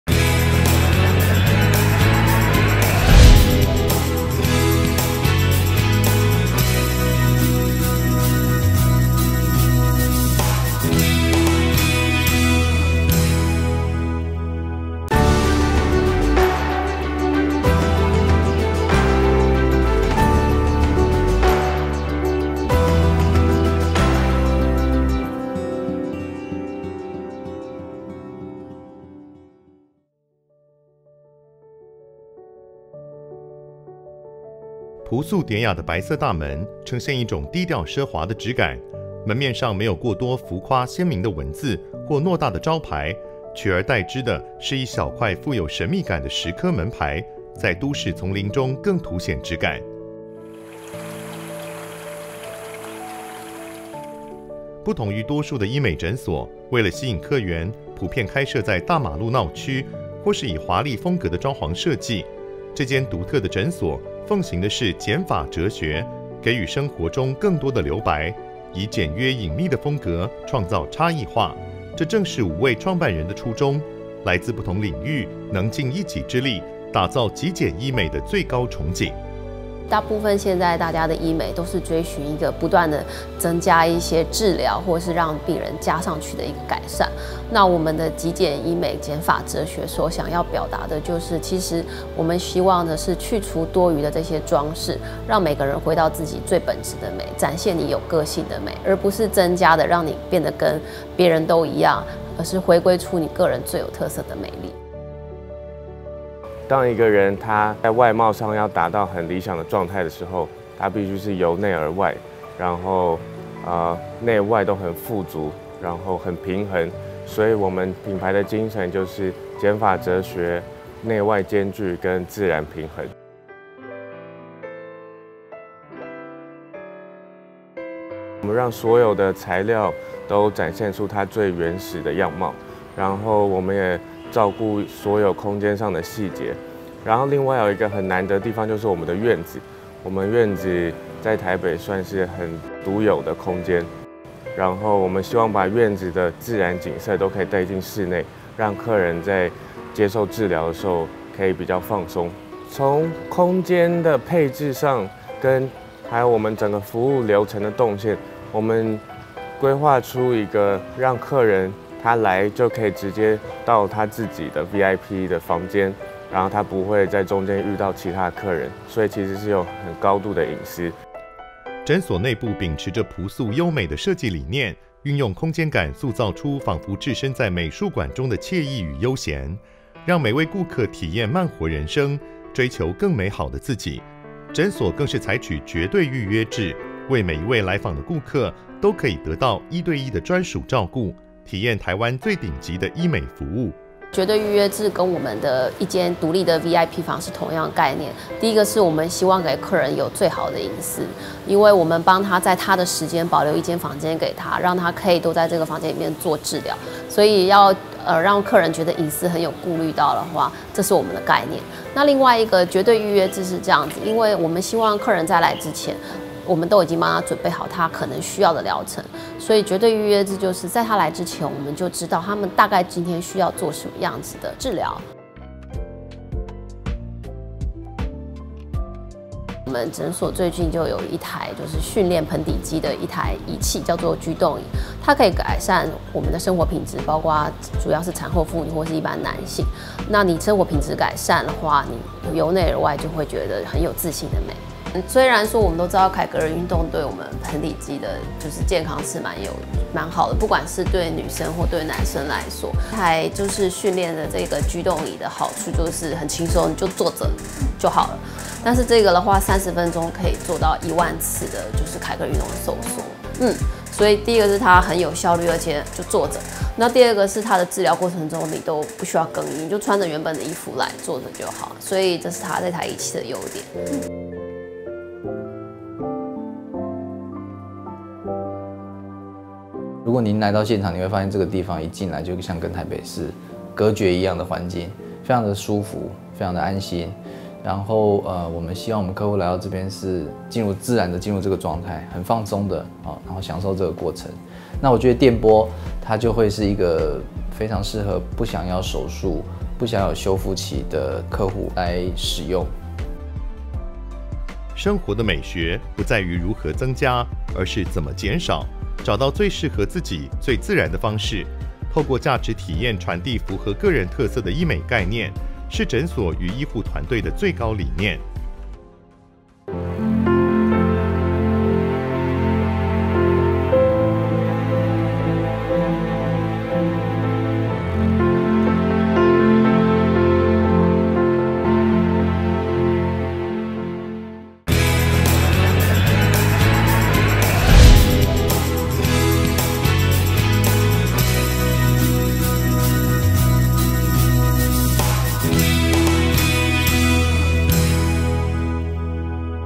台灣大未來 漫雲思境
不同於多數的醫美診所，為了吸引客源，普遍開設在大馬路鬧區、或是以華麗風格的裝潢設計。這間獨特的診所，奉行的是「減法哲學」，給予生活中更多的留白，以簡約隱密的風格，創造差異化，打造極簡醫美的最高崇景。

漫雲思境
地址:台北市大安區瑞安街61巷1弄5號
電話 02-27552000